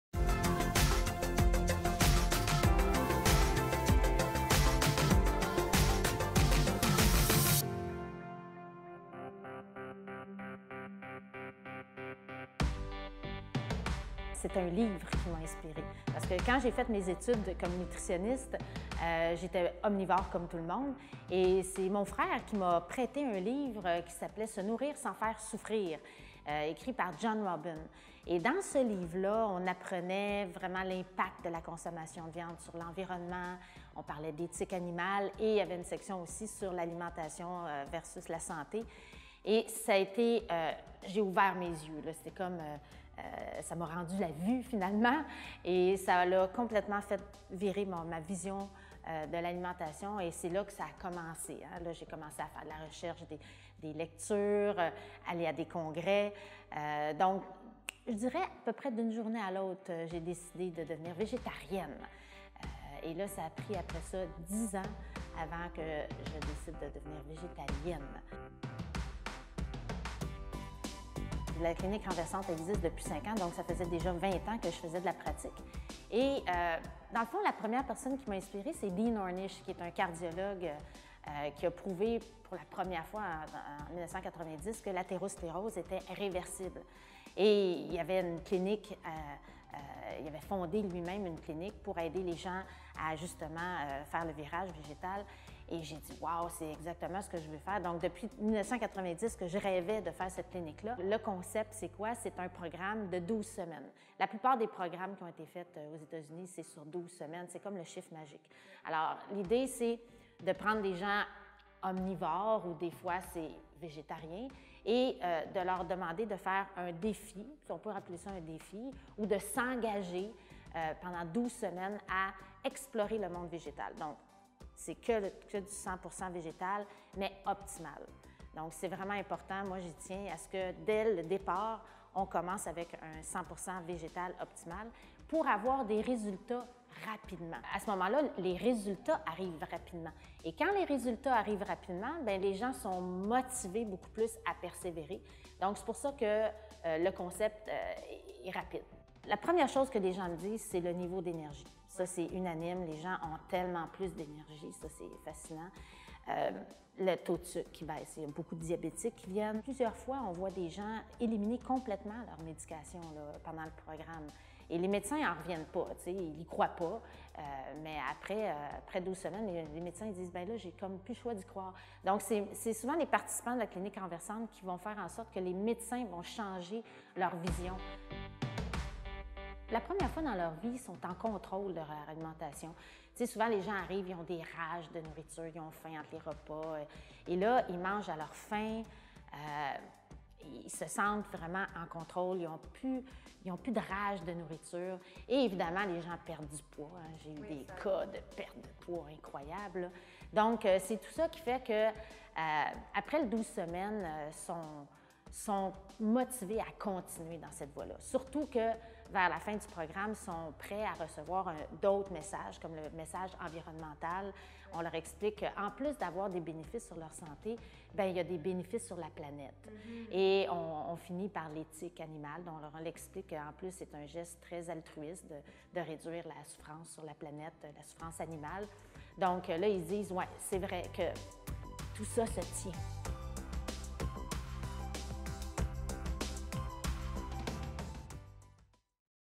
Nutritionniste qui prône une alimentation végétale
Vous avez besoin d’un petit coup de pouce ou de vous mettre au défi pour votre santé?
Découvrez Anne-Marie Roy, une diététiste-nutritionniste avant-gardiste, mobilisatrice, passionnée et fondatrice de la Clinique Renversante!

Faites l’expérience d’un défi guidé par des professionnels et des nombreux bienfaits issus d’une alimentation à base de végétaux!